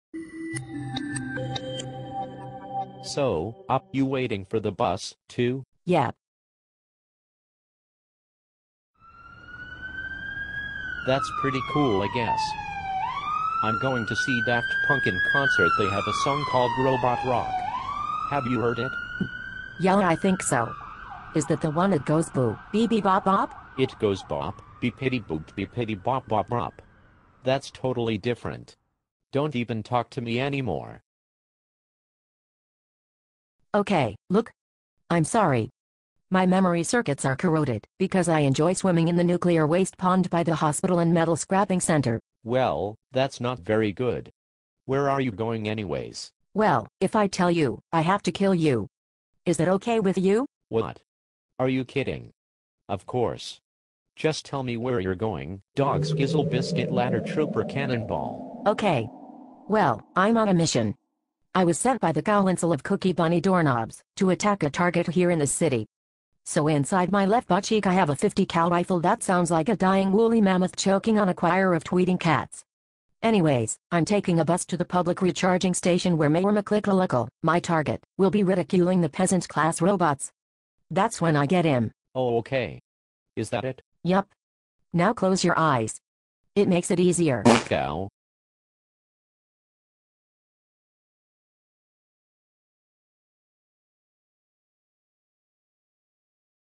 waiting for the bus
two robots wait for the bus.